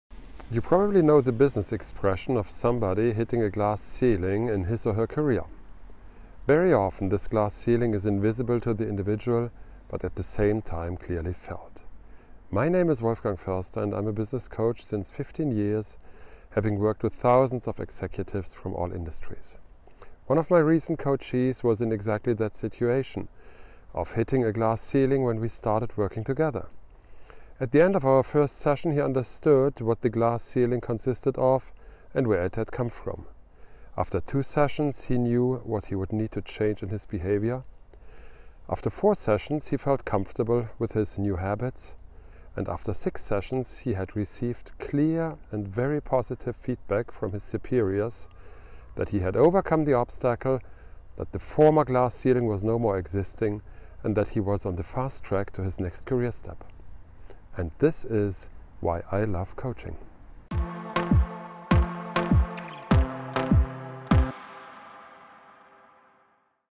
Shattering The Glass Ceiling With Coaching
As one of our most experienced coaches, Wolfgang Foerster took a moment to talk about a common obstacle encountered by most people in their careers: the glass ceiling.

The concept of a glass ceiling is something that is often remarked upon within the world of business. While more commonly used in feminist thought to describe a level of discrimination that women may face in the workplace, the term can also be employed more generally.

As a business coach of fifteen years, Wolfgang has taken on many students from across all kinds of industries that have experienced this problem in some form or another. He describes the glass ceiling as a phenomenon that is not always seen and recognised by the individual but is nevertheless keenly felt by those it impacts.

One such executive came under Wolfgang’s coaching was himself hitting that glass ceiling at the start of his course. After that first session the executive was able to identify that glass ceiling and some of the things that may be placing it there. By the end of the second session, he was able to also identify the changes in his behaviour that would be necessary to break through the ceiling to reach new heights.

By his sixth session, he had received clear and positive feedback from his superiors and was from there on a fast track to the next stage of his career. The glass ceiling had been shattered and was no more.

With a smile, Wolfgang expresses that stories like these are why he loves coaching – to take someone experiencing universal problems in executive careers and seeing real, tangible improvements to their prospects.

The glass ceiling is a problem that can be encountered by anyone at any stage of their career. But with the right steps and clear guidance, like literal glass it is a barrier that can be easily broken.